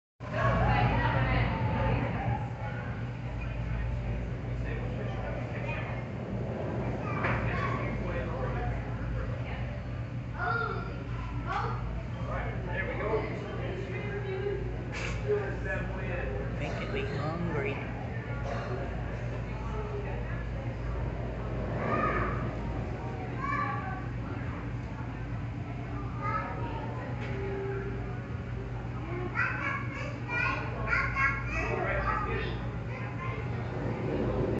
Spiny Lobsters at SC Aquarium, Charleston # aquariums
One of the many interesting creatures at the SC Aquarium, Charleston, SC. Will do a longer video showing more of the habitats and other exhibits.